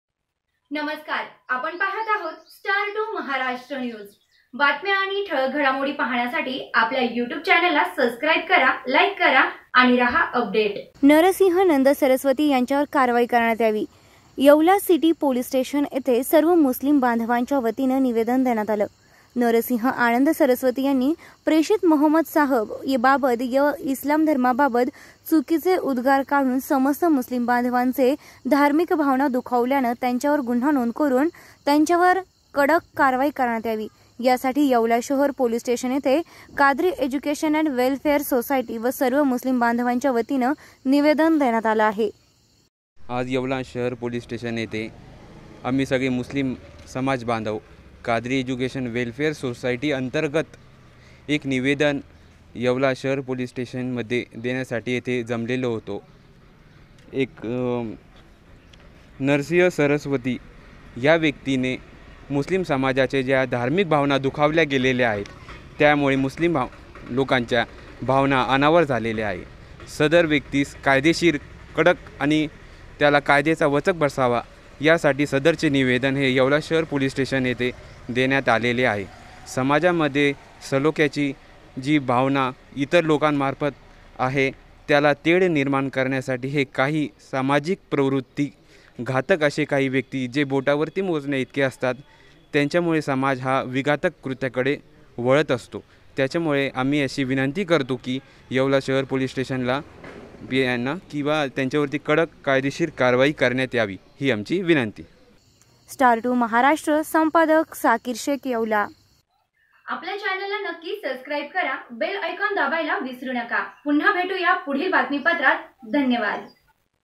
येवला । नरसिंहनंद सरस्वती यांच्यावर कारवाई करण्यात यावी पोलिसांना दिले निवेदन
Star 2 Maharashtra News